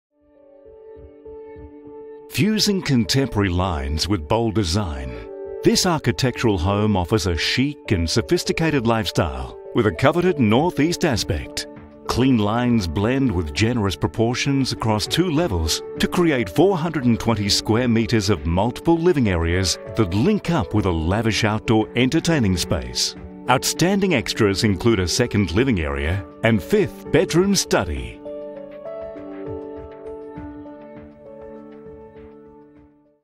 12 Milne Street, Clayfield :: Place Estate Agents | Brisbane Real Estate For Sale
Prestige Clayfield Retreat with North-Easterly Aspect

This outstanding architecturally designed family home fuses contemporary lines with bold design for a chic and sophisticated lifestyle that draws on its perfect location and coveted north-easterly aspect.

Clean lines blend with generous proportions across two oversized levels to offer an all-encompassing 354m2 floor plan that provides space for the entire family. Step into the front foyer that provides passage to open and flowing living spaces across the lower level. The finest of finishes feature throughout with lustrous tiled floors, neutral tones and the subtle finesse of modern fixtures.

Multiple living and entertaining areas integrate perfectly, while expansive sliding glass doors link the interiors to an oversized rear outdoor patio. Complete with a sparkling in-ground pool, water feature, sculptured hedges and electric vergola, this space is perfect for effortless entertaining throughout the year.

The modern kitchen overlooks the combined living and dining area, offering a flexible space purpose built for everything from simple family living to extravagant entertaining. Sleek Two Pac cabinetry contrasts against glistening black granite benches and quality stainless steel European appliances for a gourmet haven that demonstrates both style and functionality.

Four generously-sized bedrooms are positioned over the upper level - a perfect layout for growing families. This includes a massive master retreat complete with a large walk-in robe, impressive ensuite and a private balcony with adjustable shutters. The bathrooms are of the highest quality, featuring Italian ceramic tiles and black granite bench tops... finished with Villeroy and Boch tapware and accesories.

Your comfort is further assured by a second living area on this level. Other outstanding features include a fifth bedroom / study, ducted air conditioning, intercom system, fully integrated security system and built-in surround sound as well as phone/internet and cable TV's points in all the bedroom and entertainment areas.

This beautiful blue chip property is located in a jacaranda tree lined street surrounded by some of Brisbane's most beautifully established Queenslander homes. Located within the Eagle Junction State School catchment and within walking distance of St Margaret's and Clayfield Colleges, this residence also provides easy access to a sweep of nearby local amenities of Racecourse Rd, Hamilton and James St, Fortitude Valley. Public transport is close at hand; the airport link is easily accessible and the CBD is also only minutes away.

Auction In Rooms 11th December 6.30pm